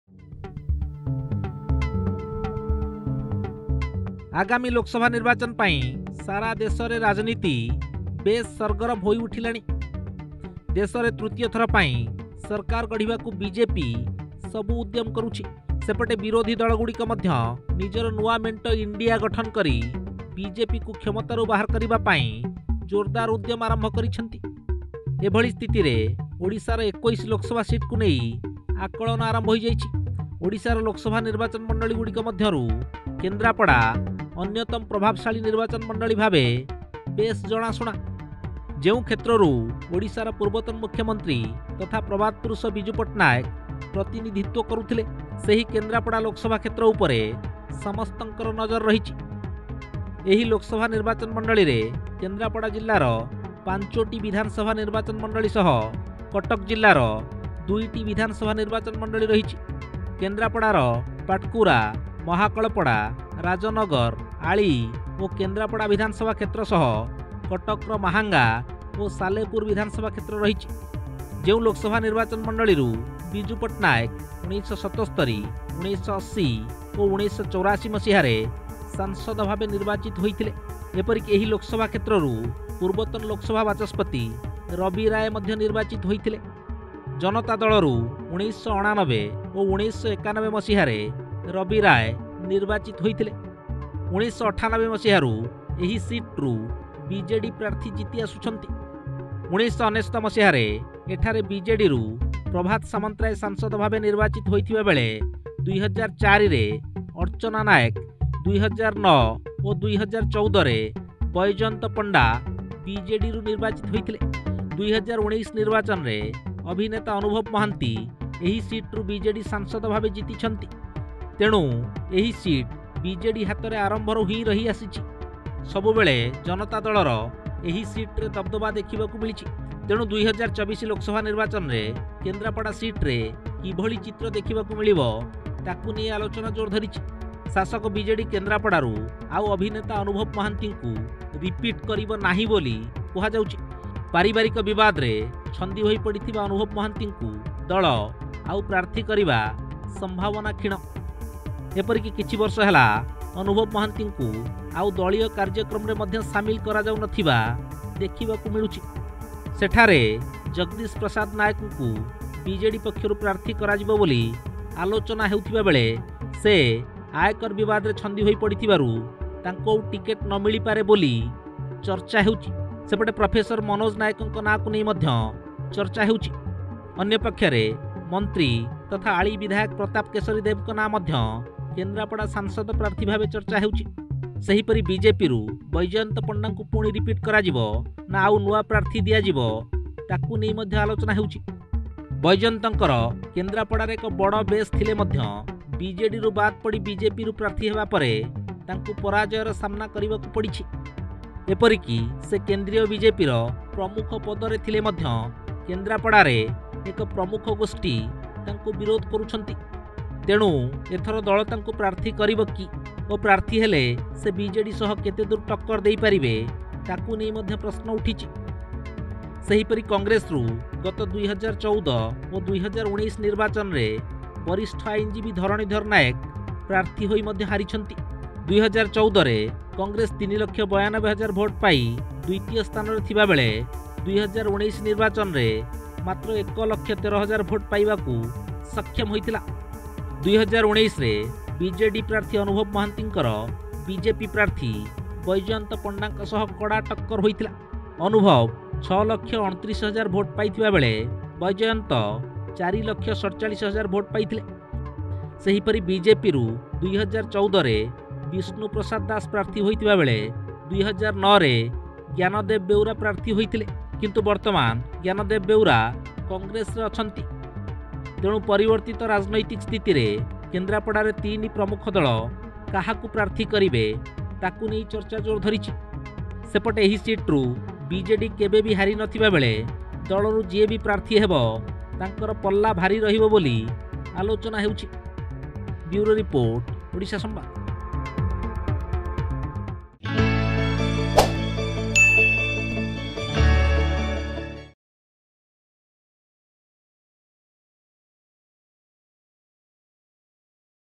Can BJP Defeat BJD in Kendrapara Lok Sabha Seat ?
ଓଡ଼ିଶାର ୨୧ ଲୋକସଭା ସିଟକୁ ନେଇ ଆକଳନ ଆରମ୍ଭ ହୋଇଛି। ଓଡ଼ିଶାର ଲୋକସଭା ନିର୍ବାଚନ ମଣ୍ଡଳୀ ଗୁଡିକ ମଧ୍ୟରୁ କେନ୍ଦ୍ରାପଡା ଅନ୍ୟତମ ପ୍ରଭାବଶାଳୀ ନିର୍ବାଚନ ମଣ୍ଡଳୀ ଭାବେ ଜଣାଶୁଣା।